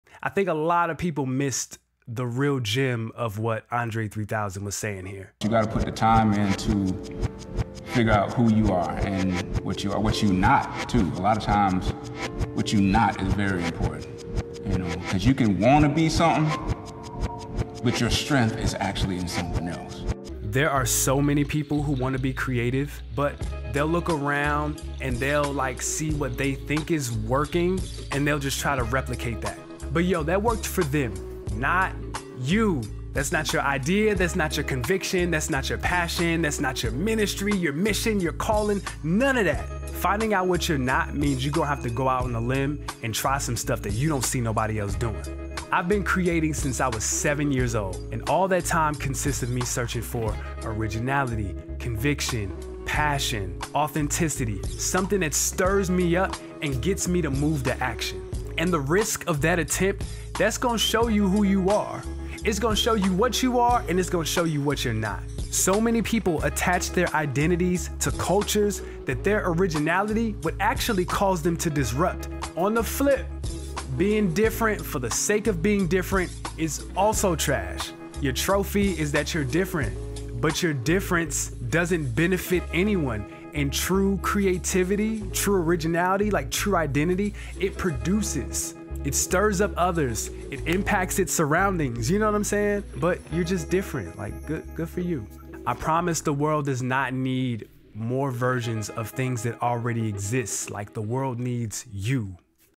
Andre 3000 & the 💎 of knowing what you're not
I think a lot of people missed the 💎 @andre3000 shared in this recent clip with Supreme. & it wasn’t him talking about needing to create either.